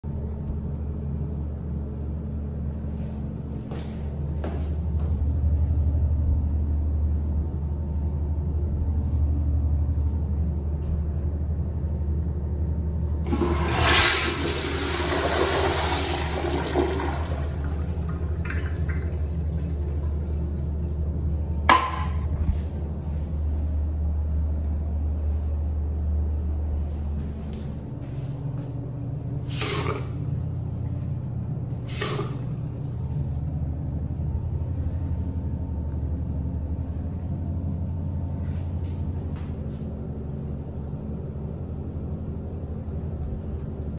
109. Safeway Unisex Restroom Full Shoot
This was filmed at Safeway in Sterling, VA.
Fixtures:
1 Crane Hymont
This restroom is pretty good 7.8/10